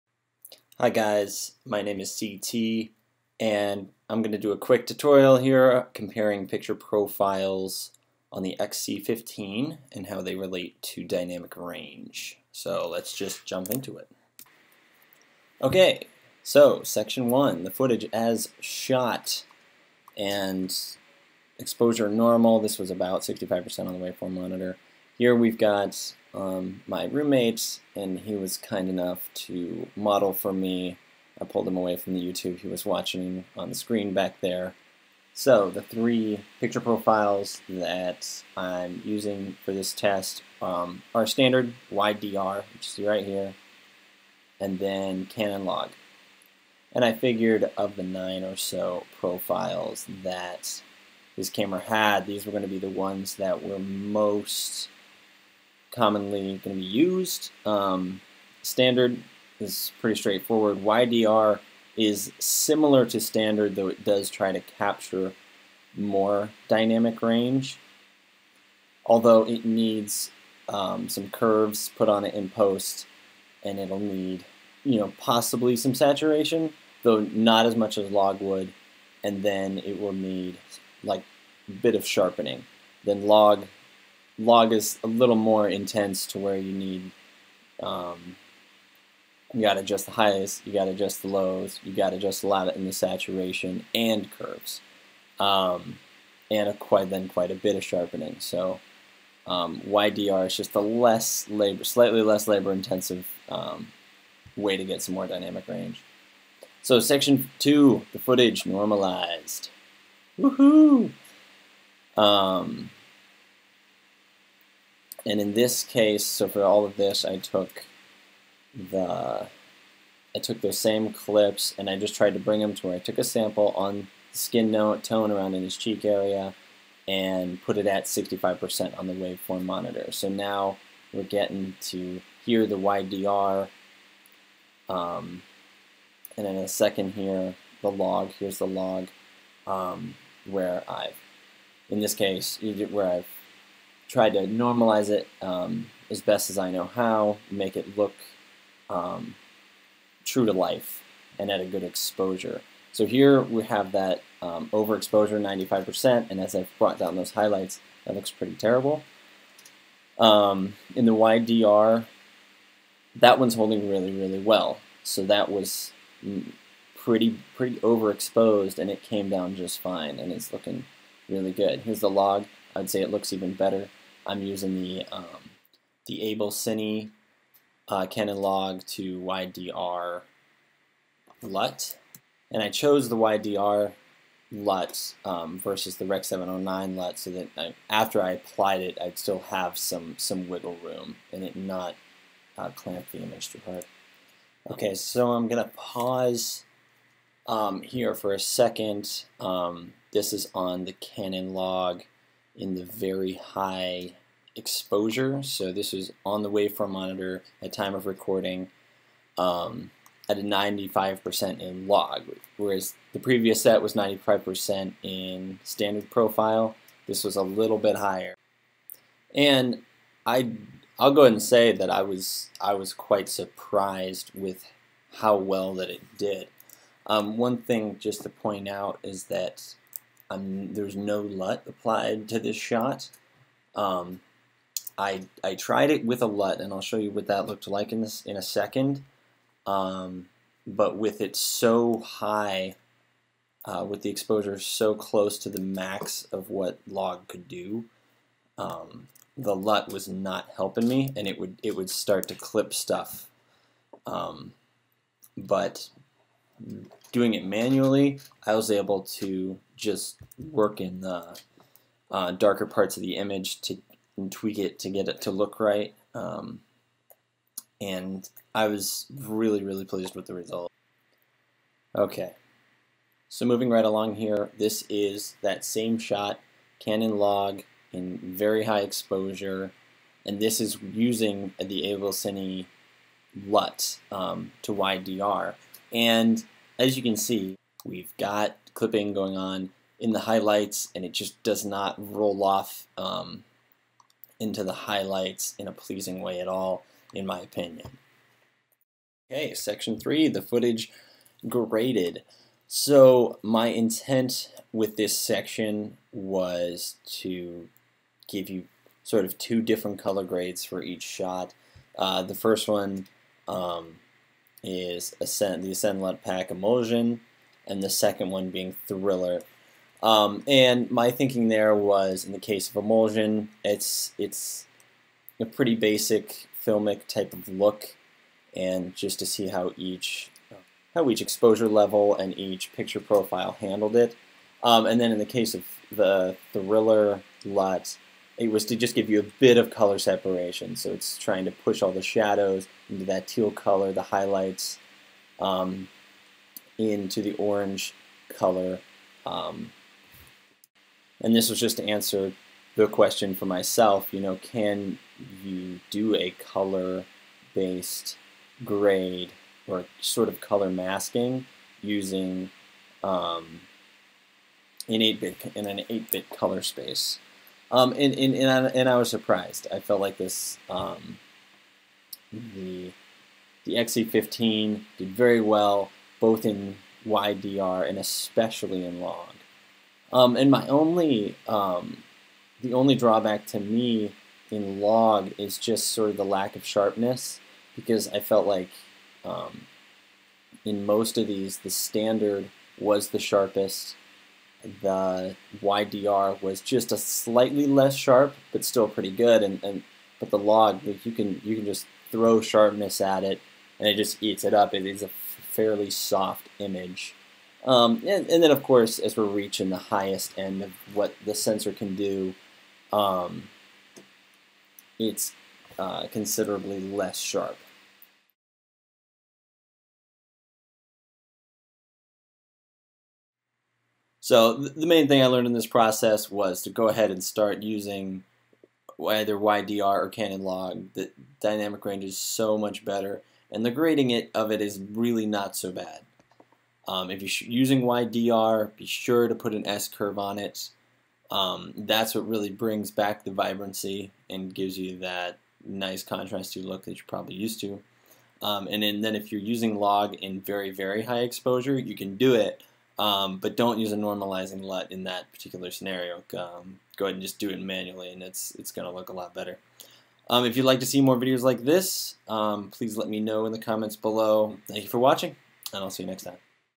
Canon XC15 Picture Profiles and Dynamic Range Test!!
Profiles tested: Standard, Wide DR and Canon LOG